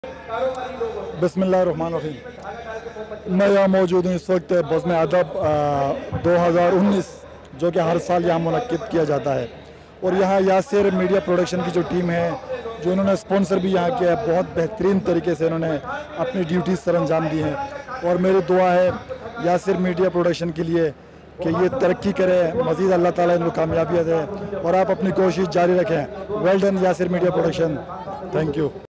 Imran khan at bazmeadab and give his reviews about about Yasir Media Production
cinematography & photography
Yasir Media Production
full entertainment
bohat maze ki hain
bhorasa karain